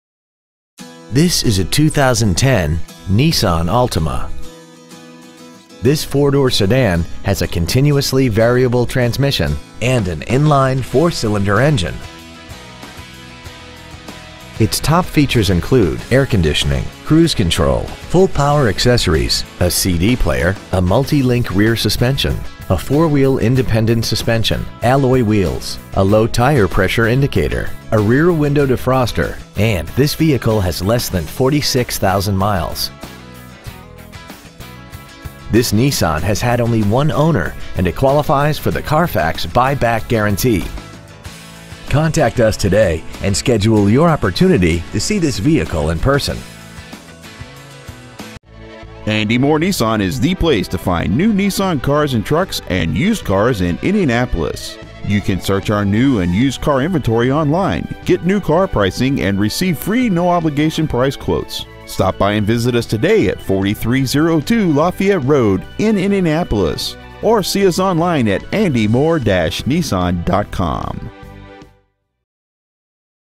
2010 Nissan Altima Indianapolis IN 46254
Year : 2010
 Make : Nissan
 Model : Altima
 Engine : 2.5L I4
 Trans . : CVT
 Exterior : Silver
 Miles : 45,693
 Interior : Charcoal
 Stock : NP3741